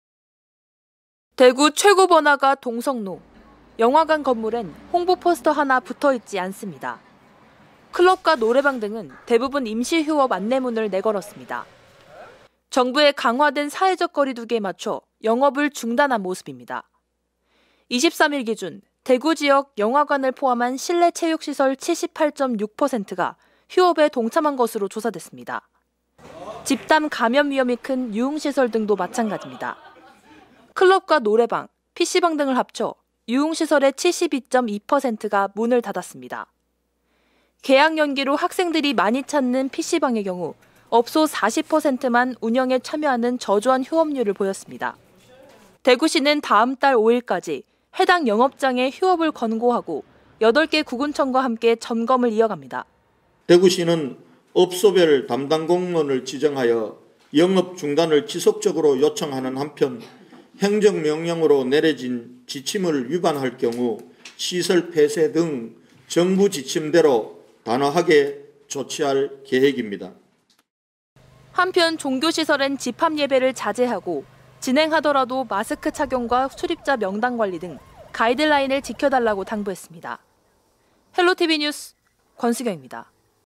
강화된 ′사회적 거리두기′...대구는 ′휴업 중′
다음 달 5일까지 강력한 ′사회적 거리두기′ 운동이 이어집니다. 대구는 실내 체육시설과 영화관, 유흥 시설 열에 일곱 여덟 곳이 휴업에 들어갔습니다.